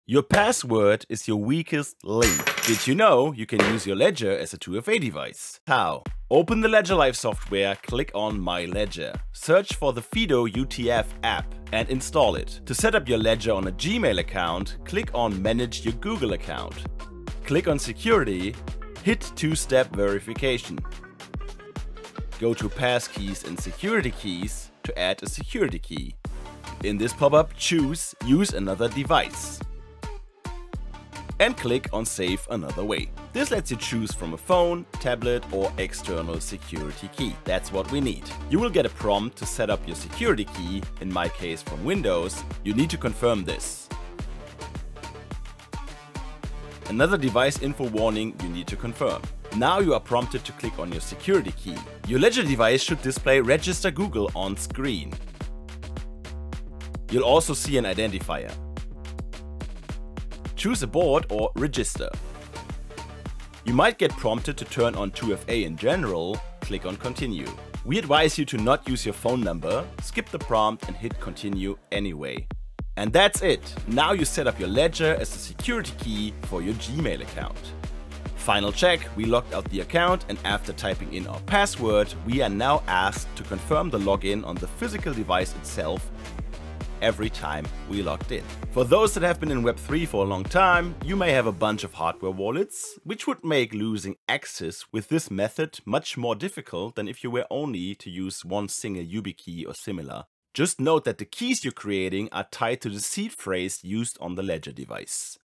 How to: Use a Ledger as a 2FA Security Key
Want to upgrade your security? Learn how to use your Ledger hardware wallet as a 2FA security key for ultimate protection against phishing and hacks! In this video, we guide you step-by-step through setting up Ledger as a security key for Google accounts, ensuring that only you can access your funds and sensitive data. Unlike SMS 2FA, which is vulnerable to SIM swaps, this method leverages your Ledger's secure chip for maximum safety. Watch now to fortify your crypto security and keep your accounts safe!

By now, you should realize how important 2FA is. If you don't, here's an in depth two-factor authentication article.

And come say Hi in our Discord